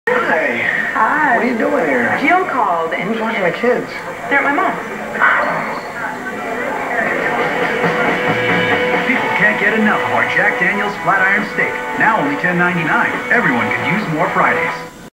CMGUS VCR CLASSIC COMMERCIALS: 2005 DEC 11 FRIDAY'S JACK DANIELS GRILL STEAK EVERYONE COULD USE MORE
CMGUS VCR CLASSIC COMMERCIALS: 2005 DECEMBER 11 FRIDAY'S JACK DANIELS FLAT IRON GRILL STEAK EVERYONE COULD USE MORE COMMERCIAL SHOWN DURING THE FOX NFL SUNDAY GAME MINNESOTA VIKINGS DEFEATED THE ST. LOUIS RAMS 27 TO 13